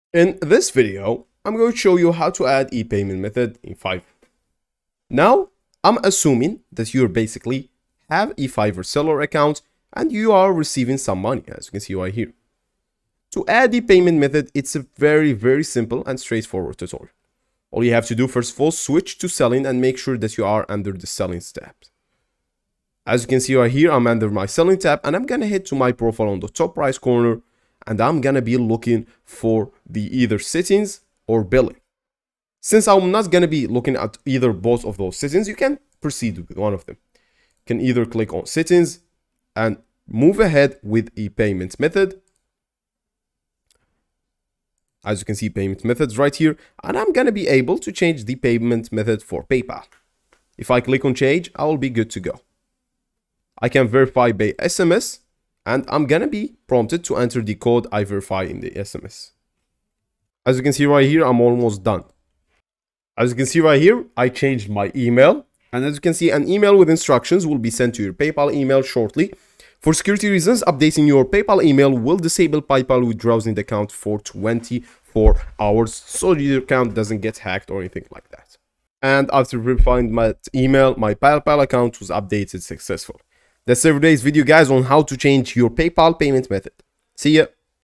How To Change Paypal Account On Fiverr Tutorial
Today I show how to change paypal account on fiverr,fiverr payment method